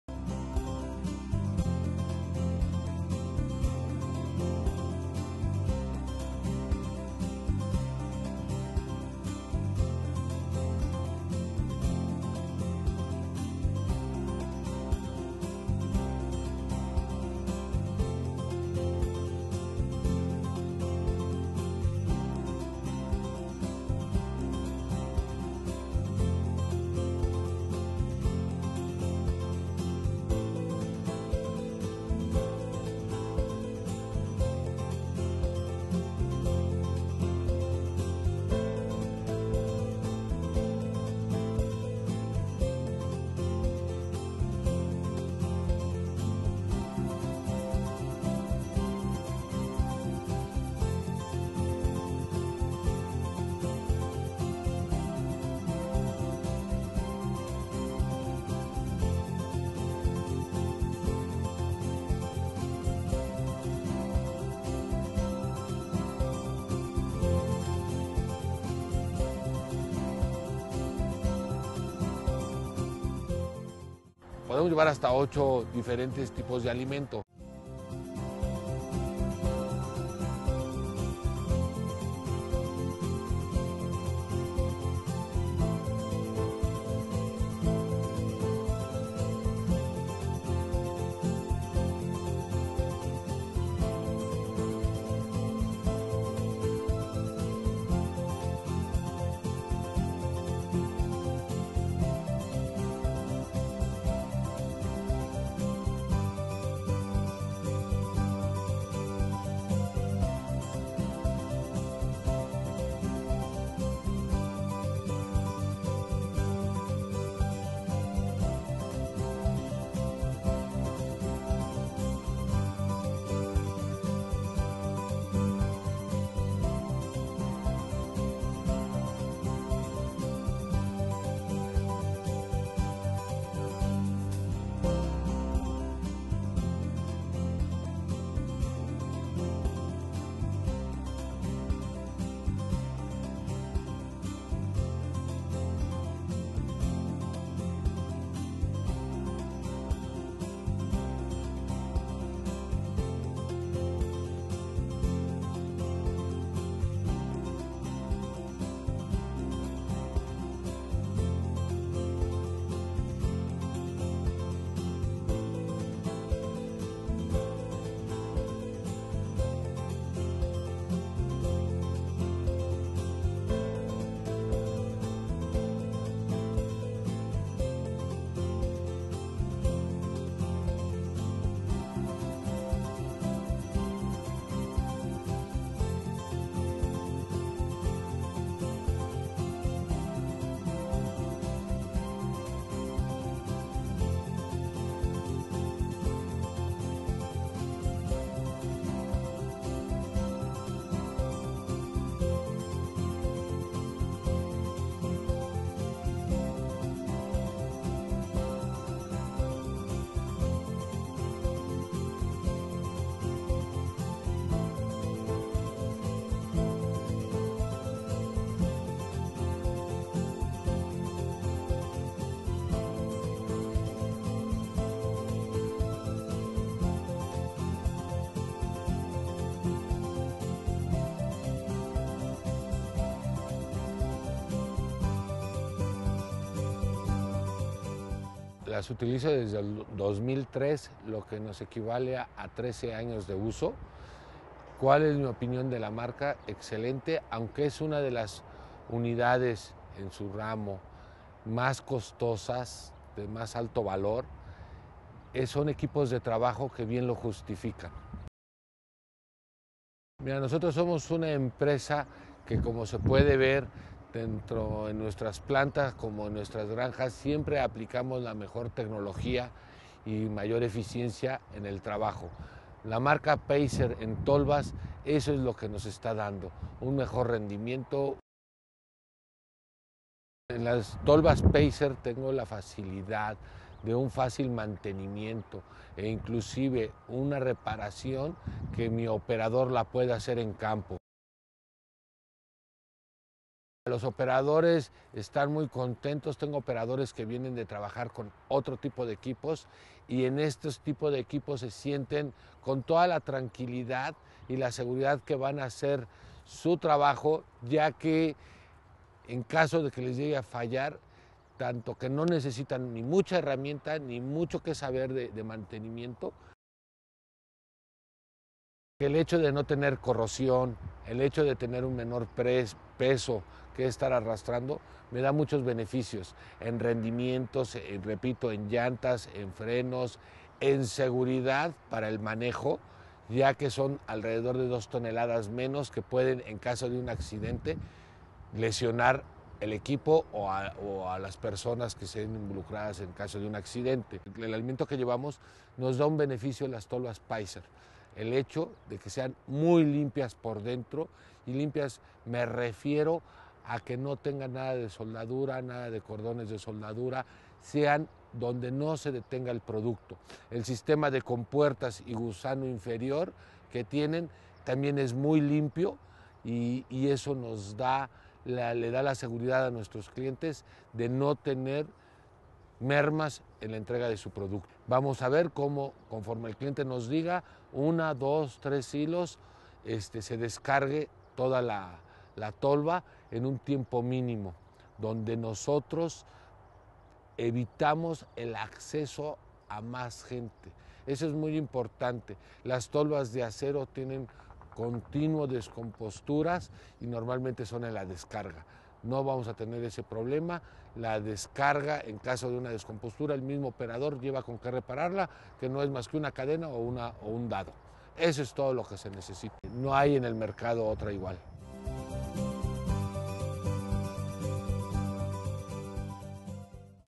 Porque usar una Tolva Pacer
Los beneficios de utilizar la Tolva Pacer sobre otras marcas se ven traducidas en diversas ventajas como: menor desgaste y corrosión, mayor capacidad de carga, más ligeras, mayor tiempo de vida, menor mantenimiento y la satisfacción de los clientes durante sus operaciones diarias.